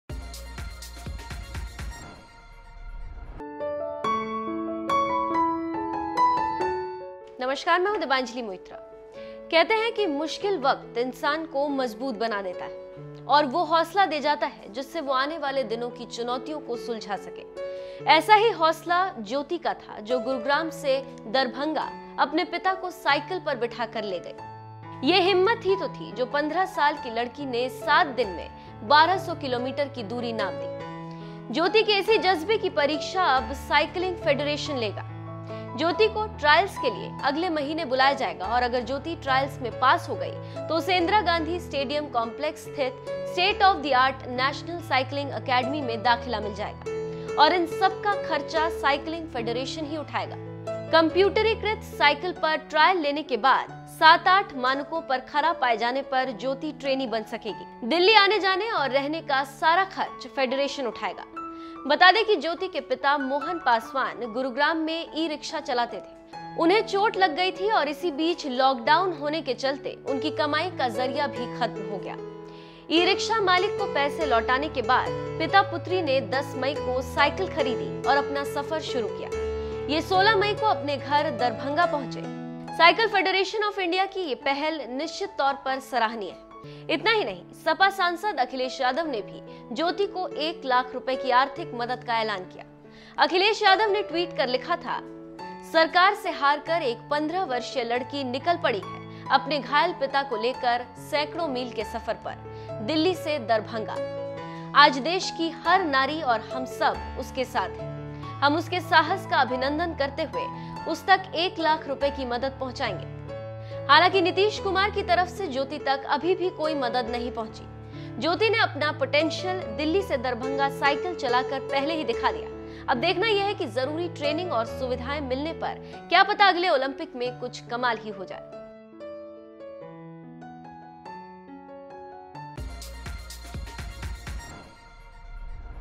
1200 किमी साइकिल चलाने वाली Jyoti को मिला जिंदगी बदलने वाली मौका
ये हिम्मत ही तो थी जो 15 साल की लड़की ने 7 दिन में 1200 किलोमीटर की दूरी नाप दी… ज्योती के इसी जज्बे की परीक्षा अब साइकलिंग फेडरेशन लेगा…  ज्योति को ट्रायल्स के लिए अगले महीने बुलाया जाएगा.